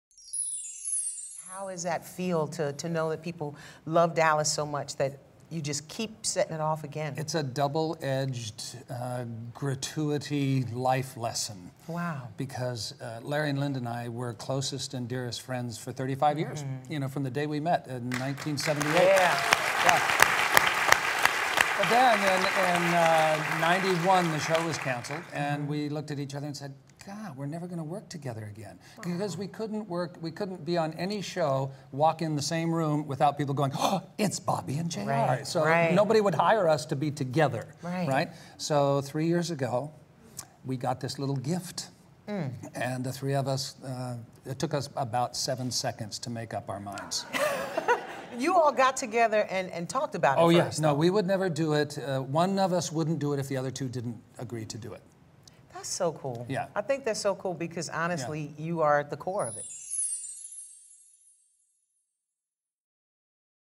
Patrick Duffy Talks About the "Dallas" Originals
Patrick Duffy talks about coming back to "Dallas" and what it took to get the three original cast members to sign on.